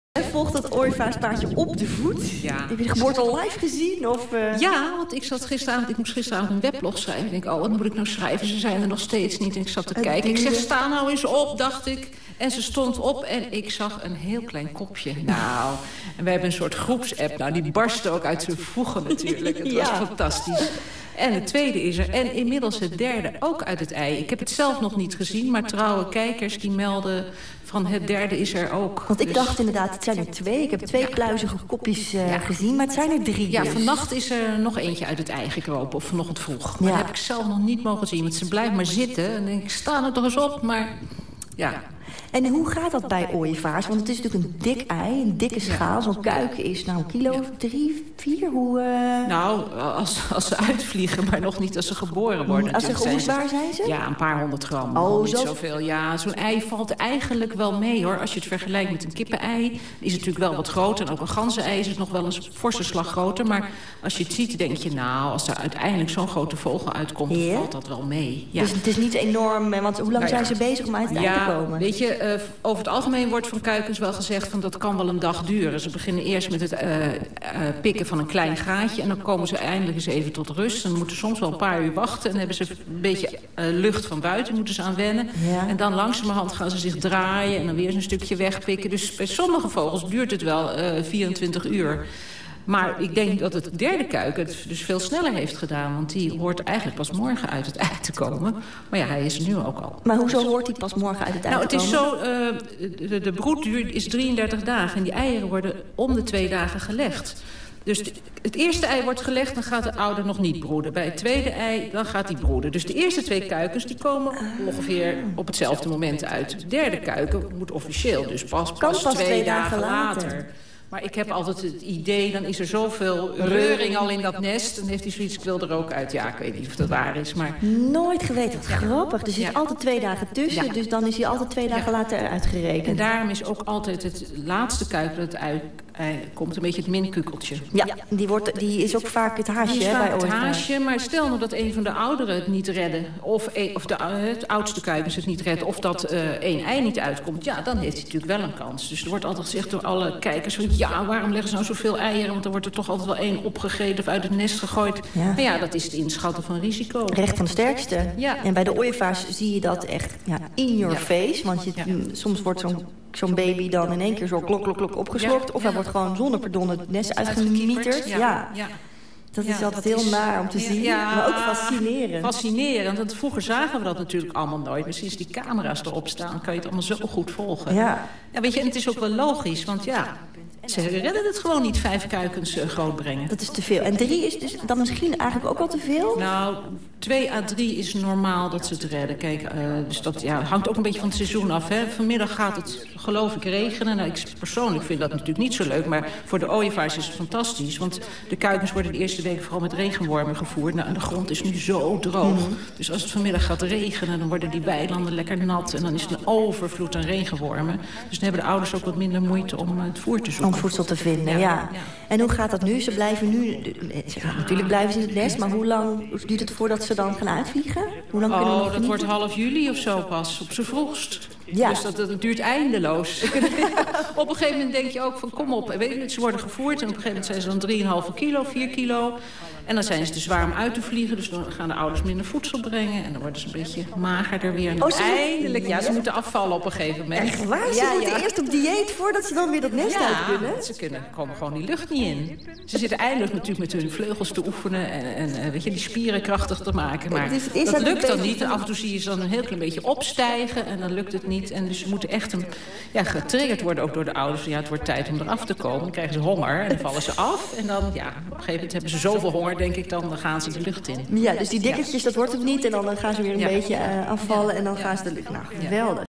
Drie jonge ooievaartjes in Gennep
Ooievaars op nest geclipt met Arda van der Lee in vroegevogels.